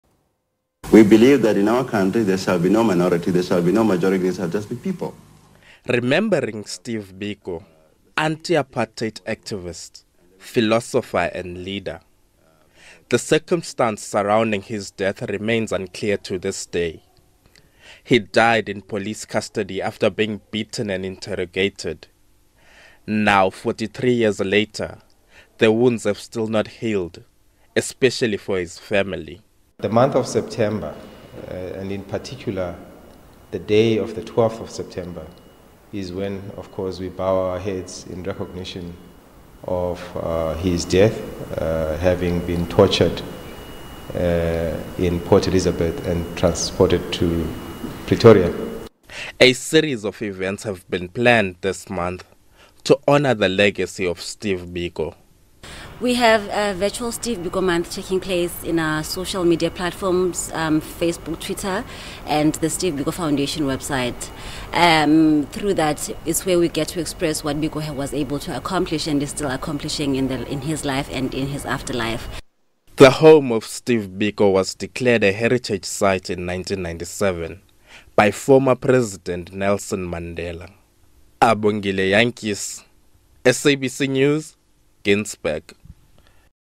Remembering Stephen Bantu Biko 43 years on
The name Stephen Bantu Biko still resonates globally!
But the man and his ground-breaking ideas had humble beginnings in the Eastern Cape where he was born and raised. The man and his conscious were formed on the dusty streets of Ginsberg in King Williamstown, where he now lies buried. He played a vital role in the struggle against apartheid, but also for the people of Ginsberg, in life and now in death.